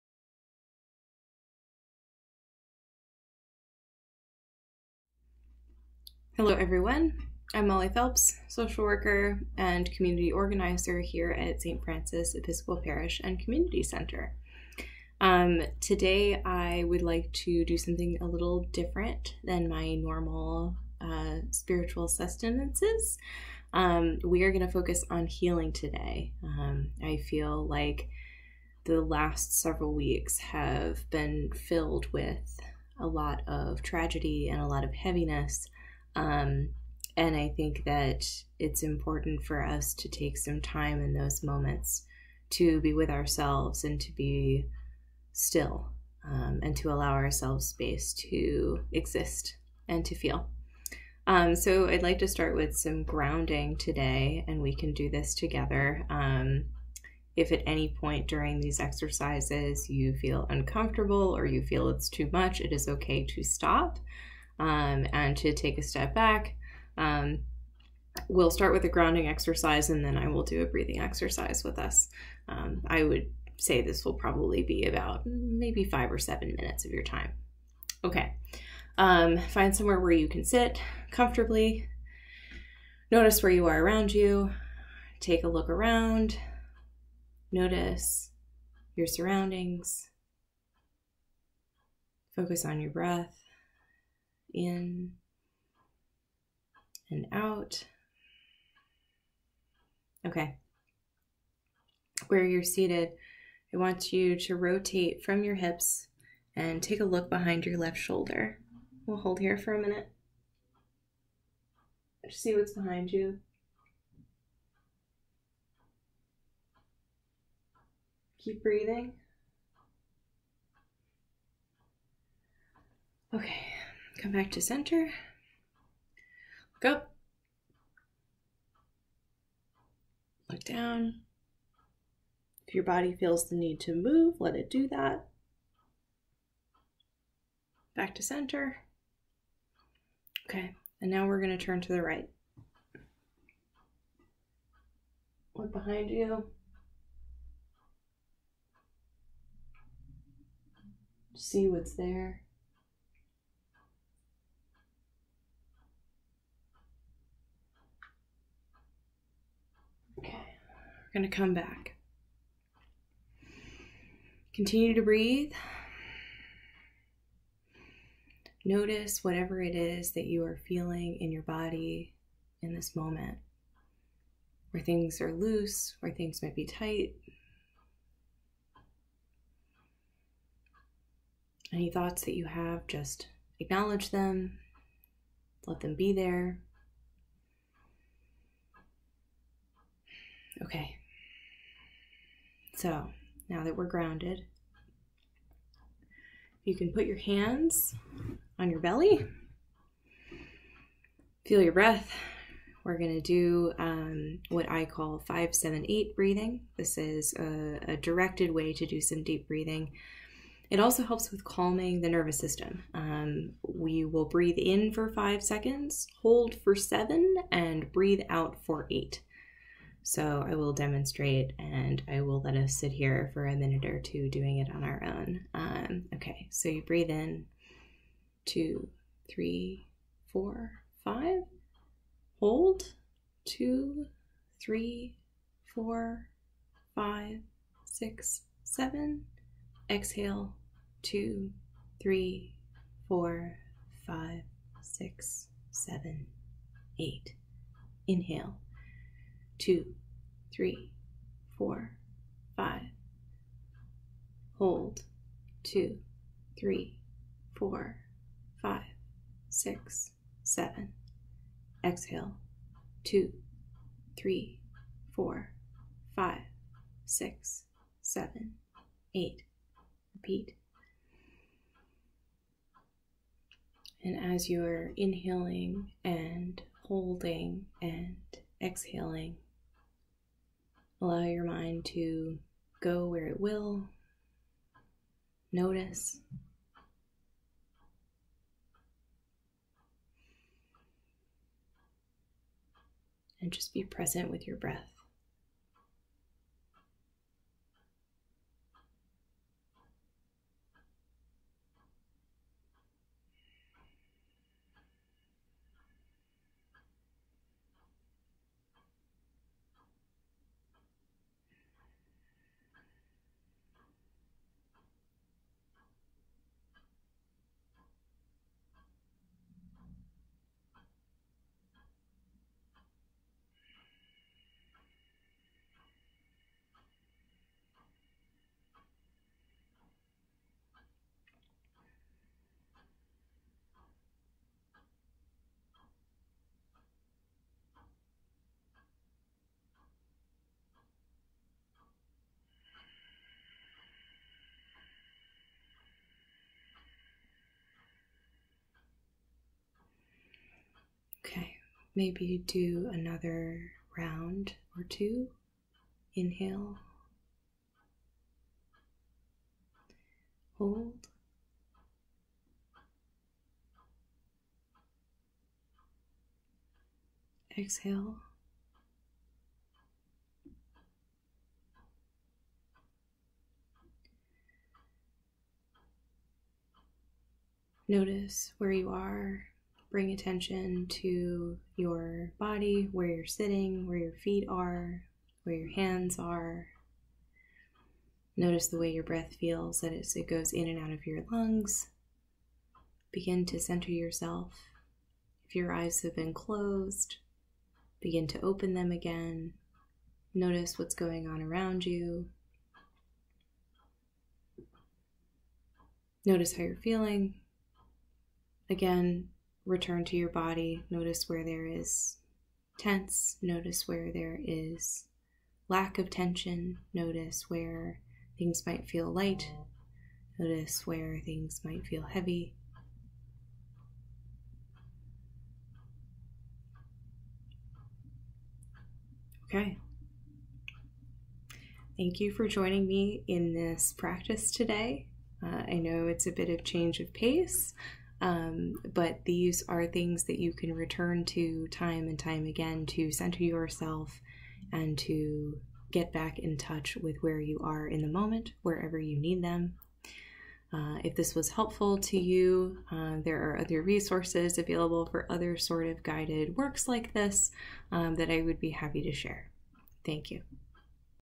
SSS June 3 2022
June 3, 2022 - St. Francis Spiritual Sustenance
with Molly Phelps, LMSW

Healing Practice with Molly

Today's SSS focuses on healing practices. Molly leads grounding and breathing exercises to help calm, center, and ground in the present moment. Take some time out of your day to pause and reflect on how your body is feeling before and after these exercises.